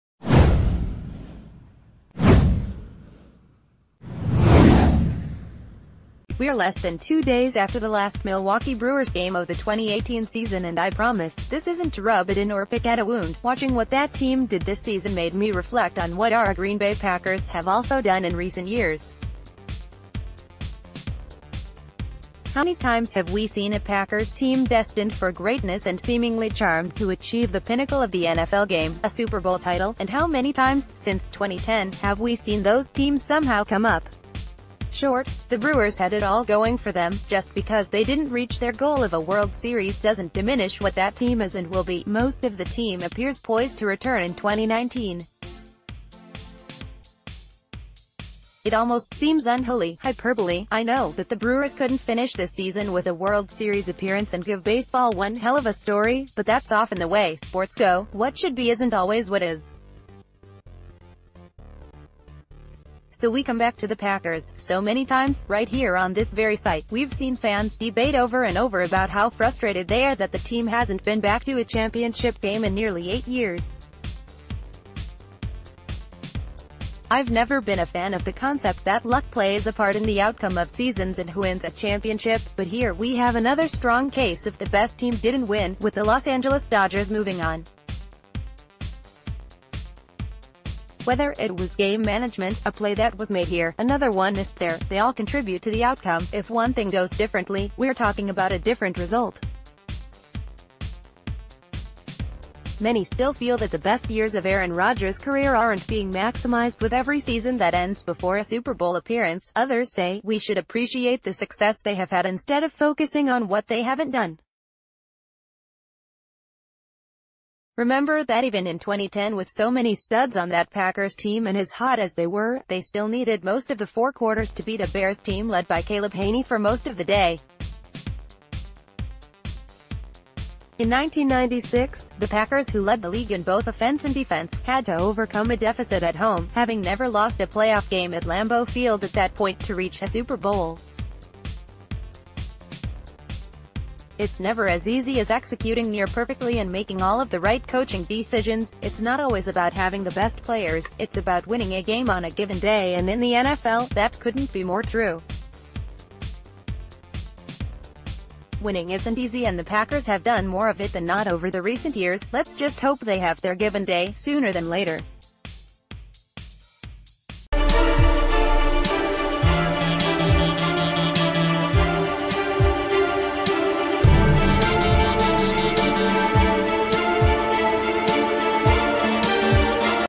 Brewers Remind Us: It's Hard to Win It All
We're less than two days after the last Milwaukee Brewers game of the 2018 season and I promise, this isn't to rub it in or pick at a wound.  Watching what that team did this season made me reflect on what our Green Bay Packers have also done in recent years. How many times have we seen a Packers team destined for greatness and seemingly charmed to achieve the pinnacle of the NFL game: a Super Bowl title?  And how many times, since 2010, have we seen those teams somehow come up short? The Brewer...